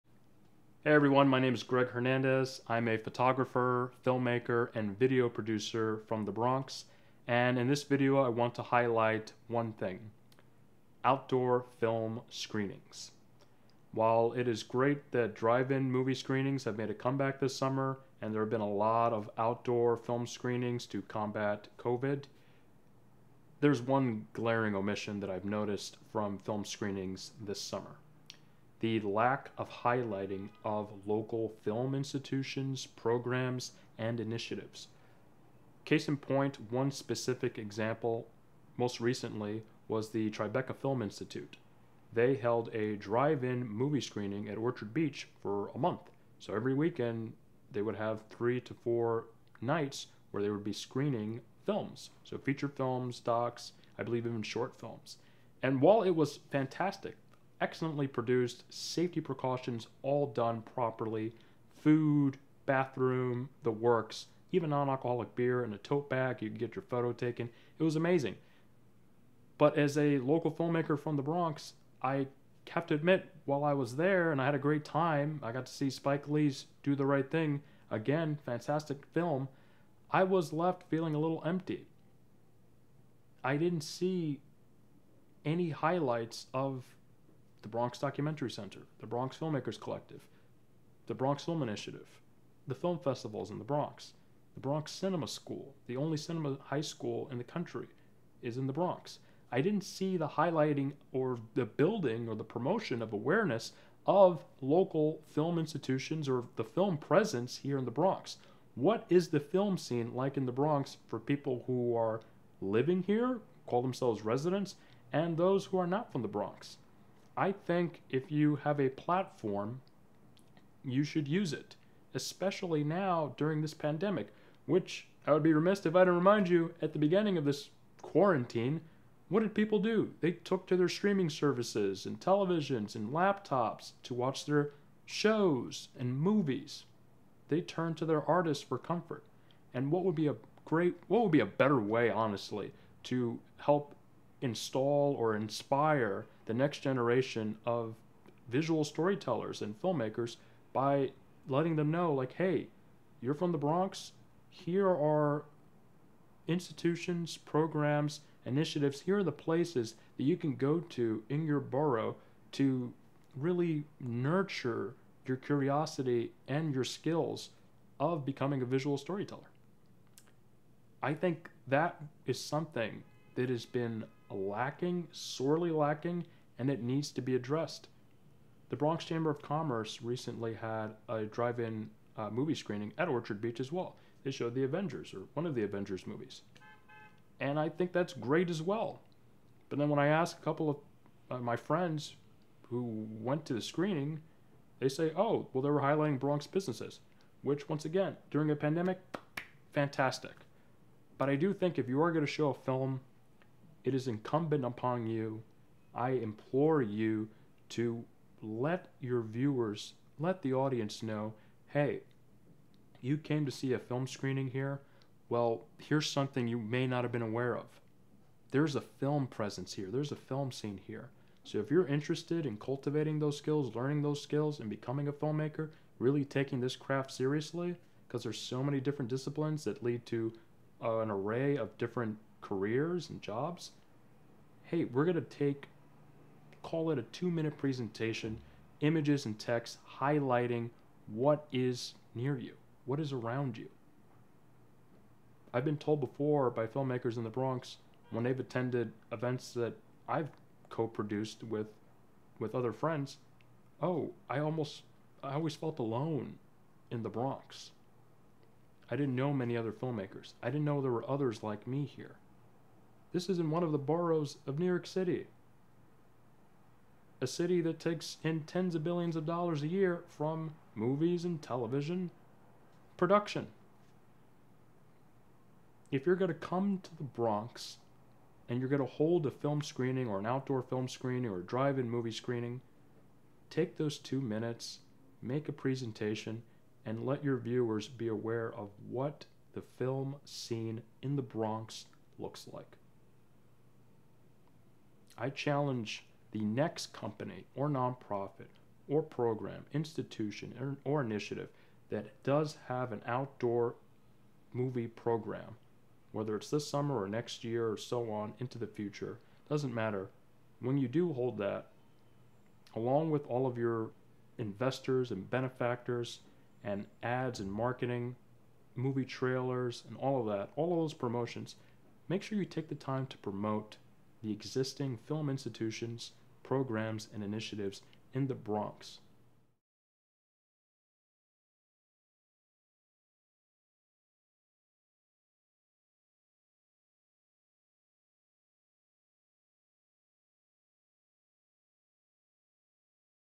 Promote your local Film Presence
With the comeback of Drive-in and outdoor movie screenings this summer, it's important to note that companies, nonprofits, and initiatives have a duty, especially during this pandemic to inform audiences of their local film presence.

This video is not meant to defame Tribeca Film Institute or The Bronx Chamber of Commerce, it is meant to highlight the light ask of promoting the local film presence in The Bronx.